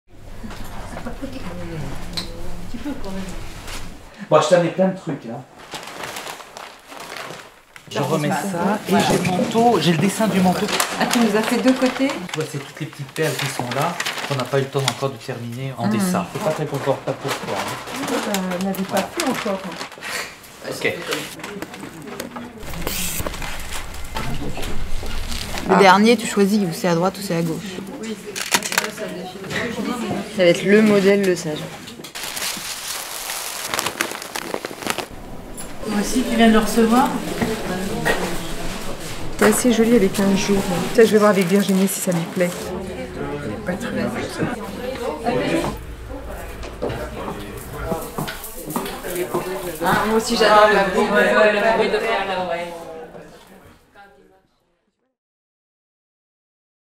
Métiers d’Art 2019/20 Show in Paris teaser – CHANEL Shows
Behind the scenes with Virginie Viard, CHANEL’s Artistic Director of Fashion collections. A creative dialogue with Hubert Barrère, Artistic Director of the embroiderer Lesage, in the lead up to the Métiers d’art 2019/20 show at the Grand Palais in Paris on December 4th.